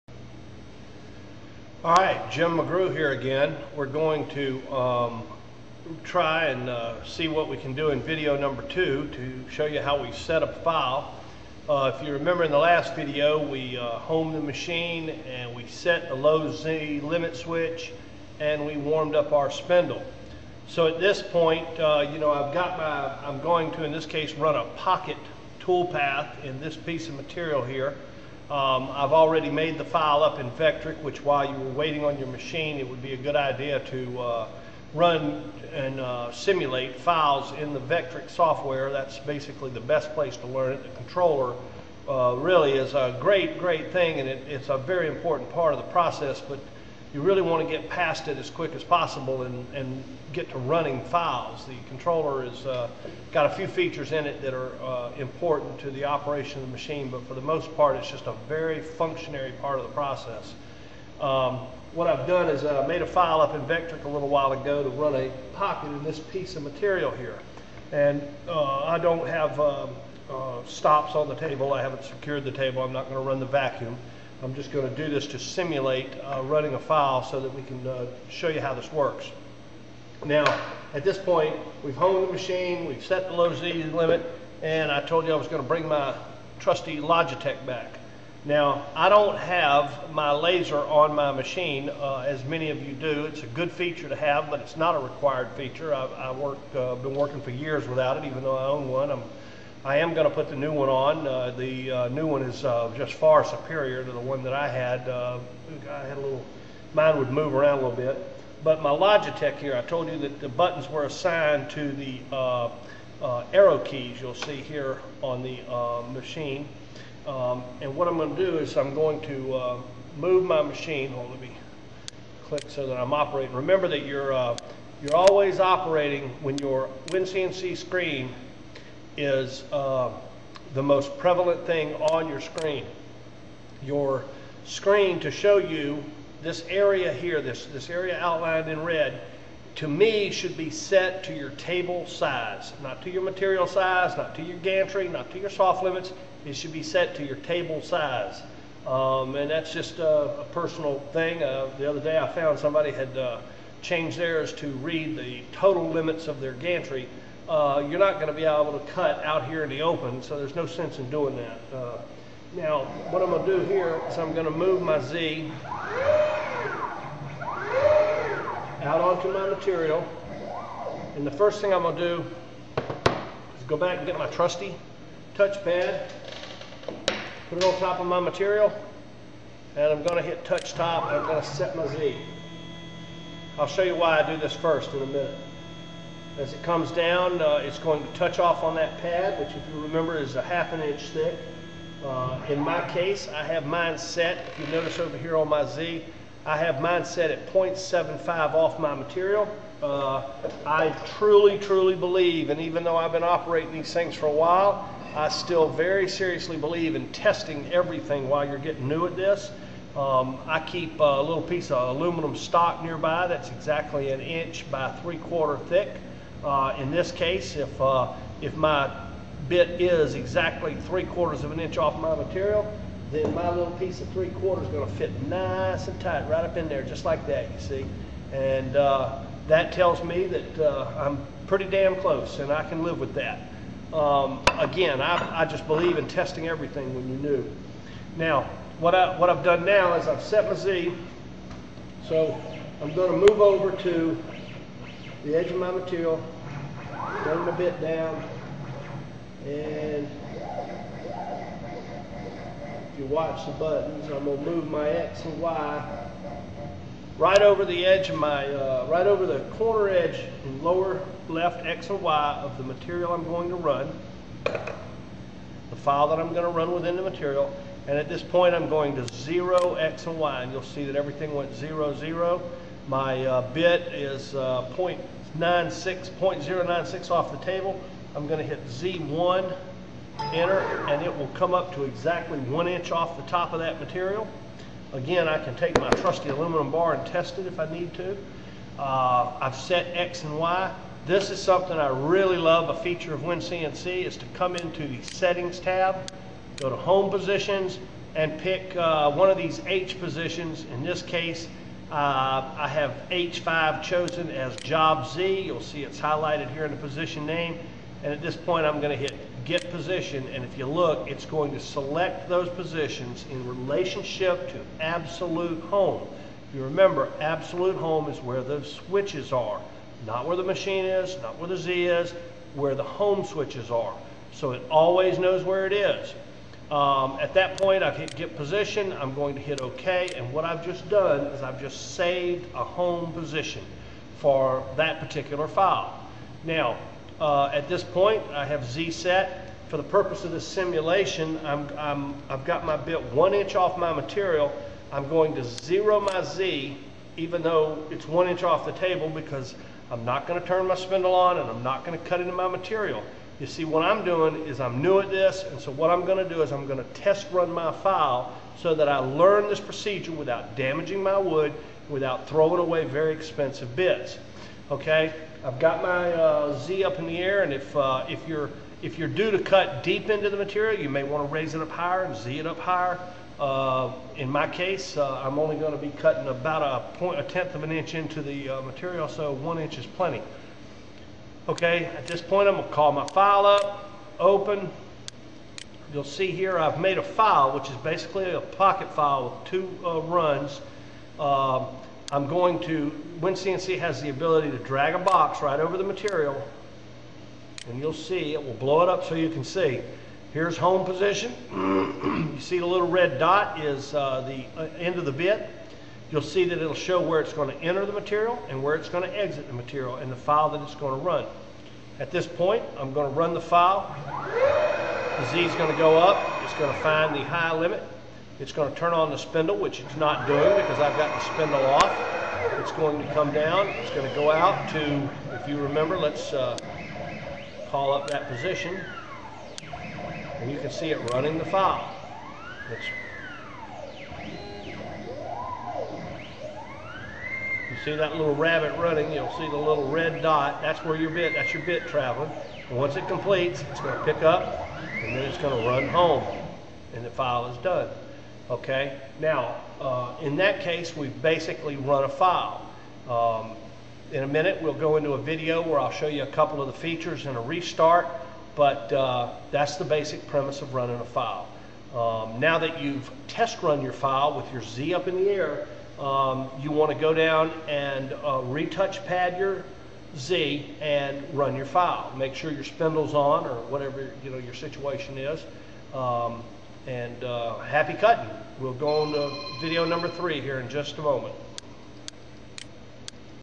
McGrew wincnc video 2.mp4
some wincnc instruction og running simple files with a CAMaster cnc machine